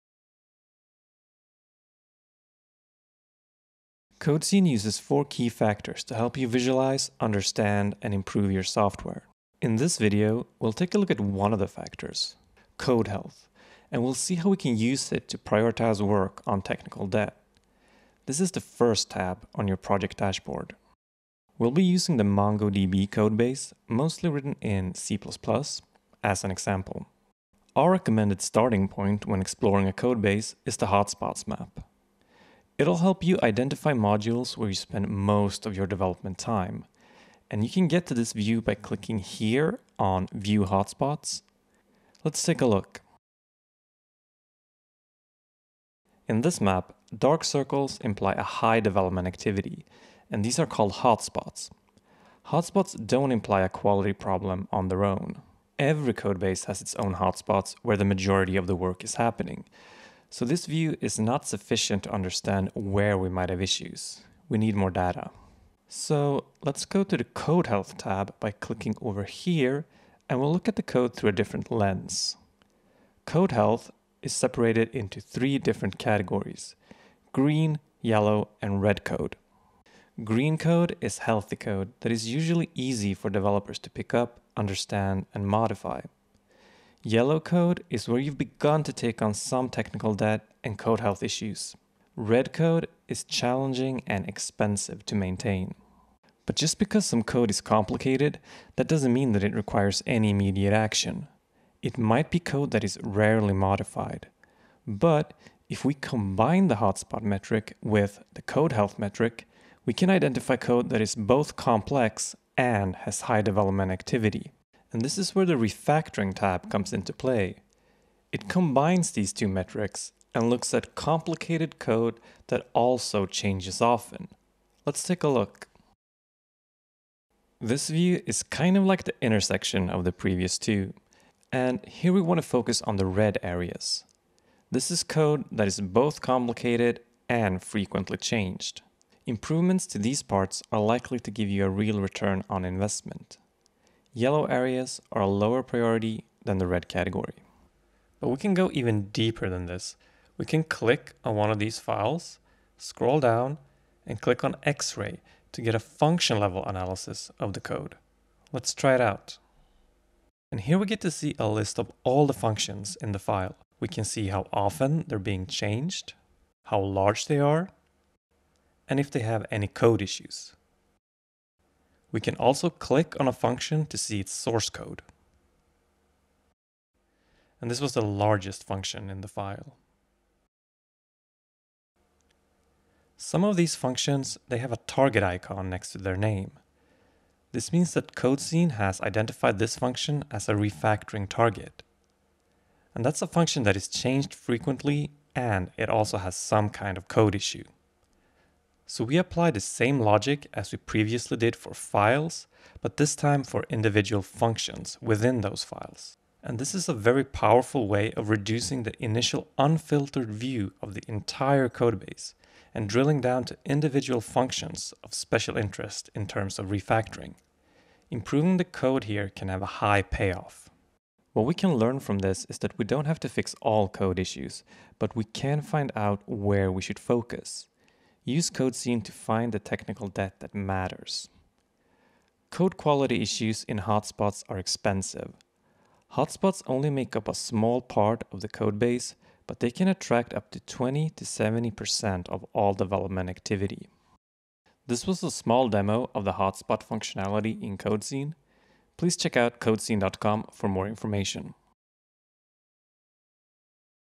Technical Debt: Identify, Visualize and Manage Based on Impact
In this tutorial you'll quickly understand how you can visualize codebases of any size and pinpoint technical debt using Hotspot Maps. You will learn how to:

- Identify modules where you spend most of your development time and where the majority of work is happening.

- Using CodeScene's CodeHealth™ Metrics you'll see code that will be separated into three different categories.

Green code:
Healthy code that is easy for developers to pick up, understand and modify.

Yellow code:
Code where you've begun to take on some technical debt and code health issues.

Red code:
Code that is challenging and expensive to maintain.

How will this help you?

- With these views on code and by looking at Refactoring targets, you'll both identify code that is complex, has high development activity and changes often.

- You'll have an understanding on where you should focus your improvements and get a real return on investment.

- You can then dig even deeper with X-Ray to get a function level analysis of the code.

- See all the functions within a file, understand how often they're being changed, how large they are, and if they have any code issues.